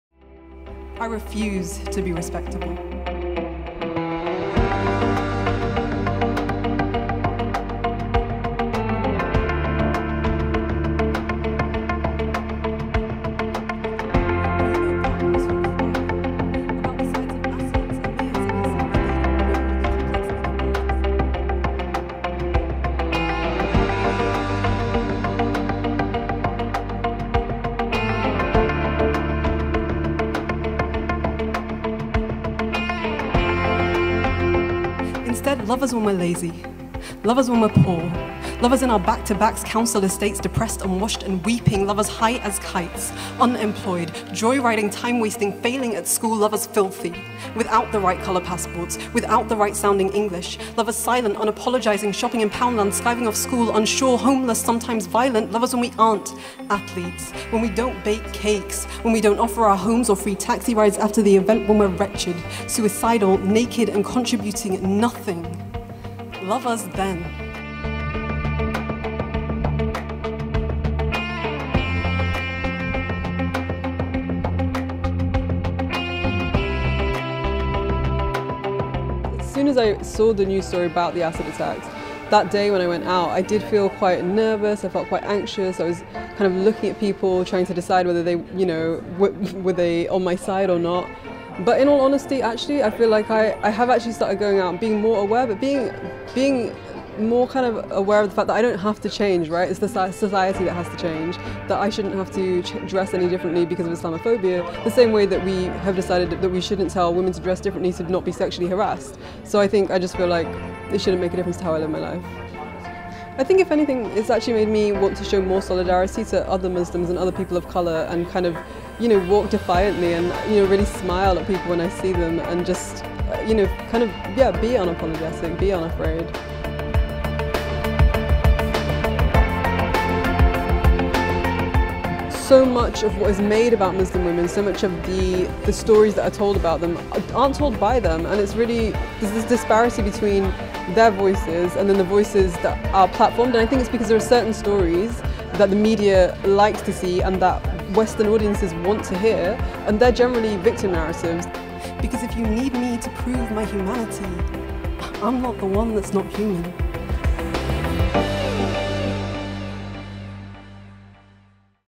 Poet Suhaiymah Manzoor-Khan is Muslim and makes no apology for it
"If you need me to prove my humanity, I'm not the one that's not human."